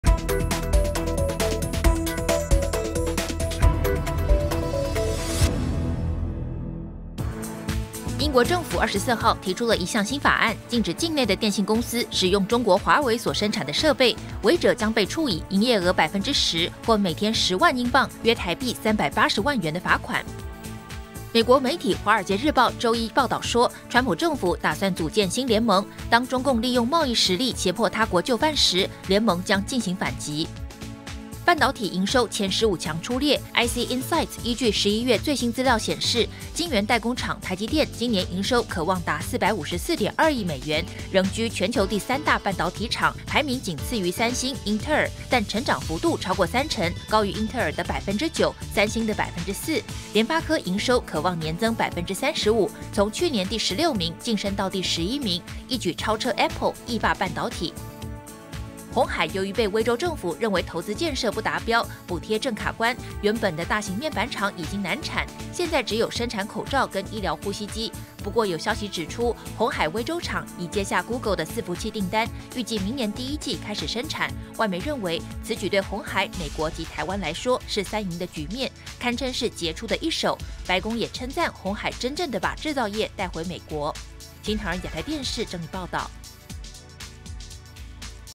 台積居全球第3大半導體廠 聯發科超車Apple｜財經100秒
🔸英國提新法案「禁用華為設備」違者得罰款
🔸傳川普政府擬組新聯盟 對抗中共貿易霸凌
🔸台積居全球第3大半導體廠 聯發科超車Apple
🔸因應市場變化 傳鴻海威州廠接下Google訂單